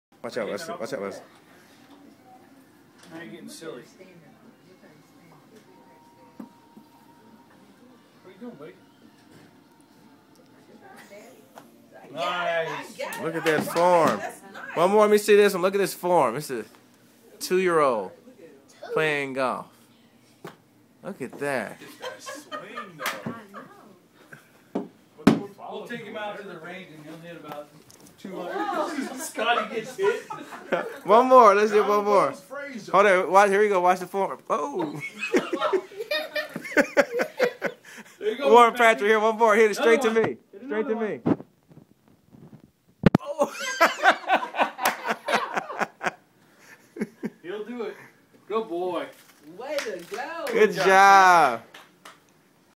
The Next Tiger Woods
This 2 year old could be the next Tiger Woods golfing sensation.